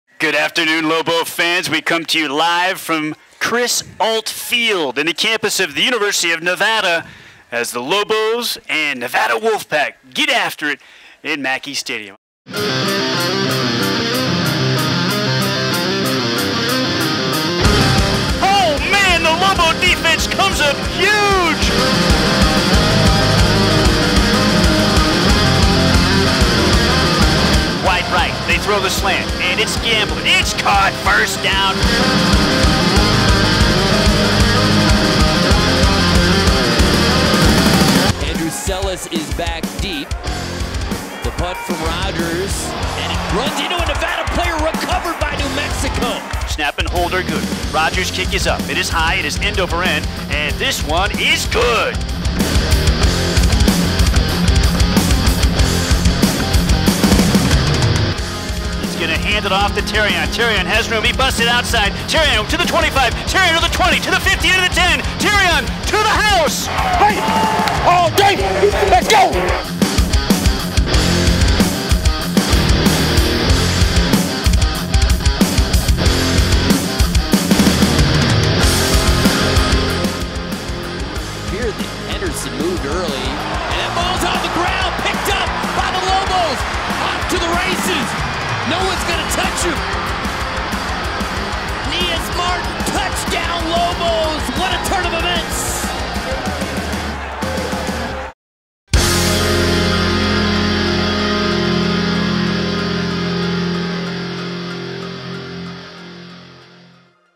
Football's Conference Record Even at 1-1 With Loss At Nevada
After battling with the Wolfpack through 4 quarters, the scoreboard was not in the Lobos' favor, as they fall t0 1-1 in the Mountain West and 3-3 overall.